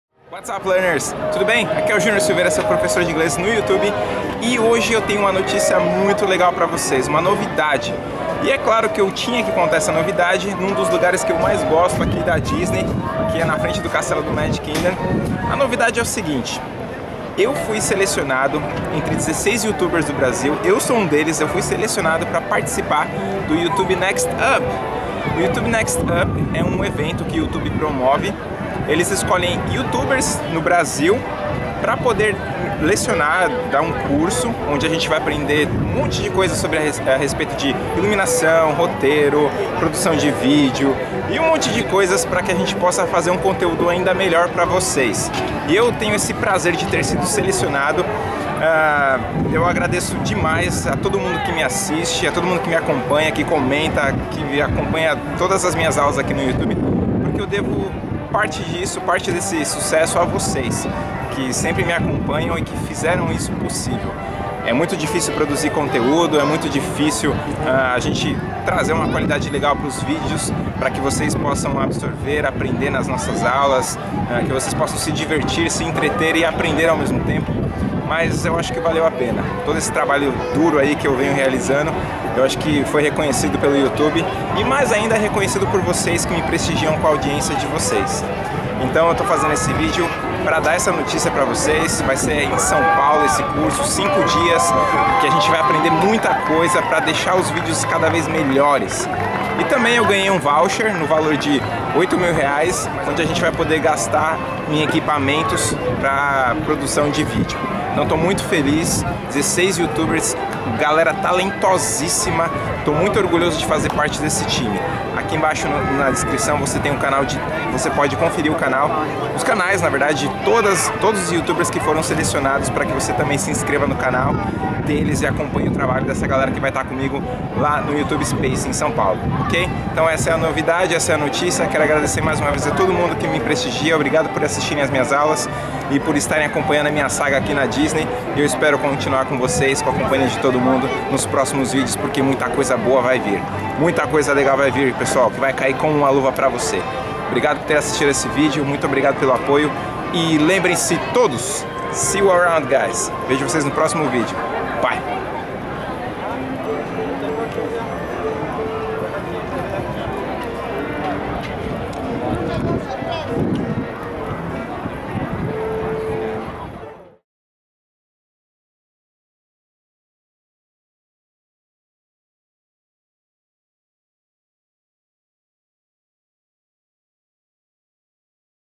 NOVIDADE: YouTube NextUp | VEDO # 27 | Junior Silveira
Eu sei que somos um canal em crescimento e que tenho muito que aprender e a melhorar aqui. Mas por tanto apoio e carinho de todos que assistem as minhas aulas, é que eu fico motivado e inspirado a produzir mais conteúdo! Estamos no NEXTUP graças a vocês!! Obrigado por tudo learners!!